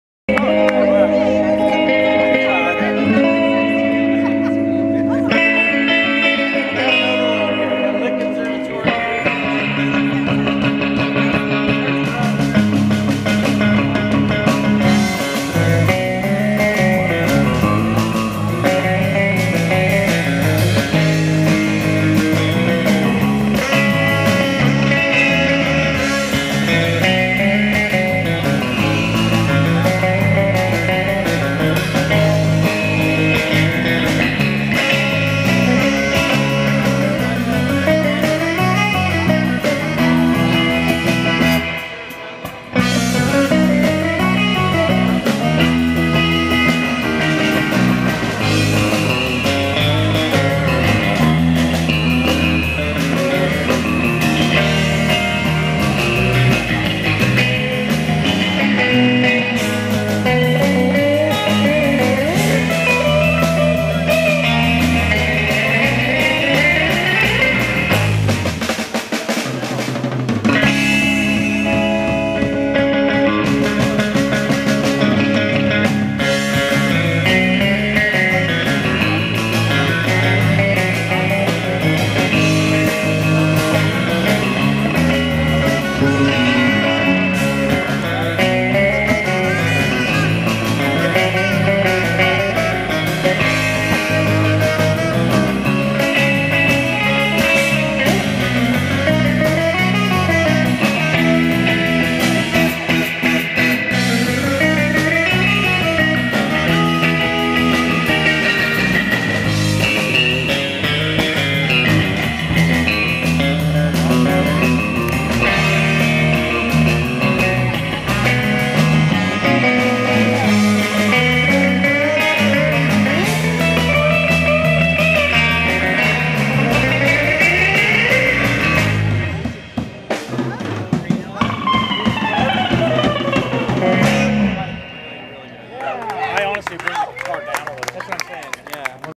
The Dead Kampers (clip #2) August 12th, 2022, Santa Cruz, Ca.
Fantastic surf band The Dead Kampers performing at Midtown Friday's Summer Block Party in Santa Cruz, California on August 12th, 2022.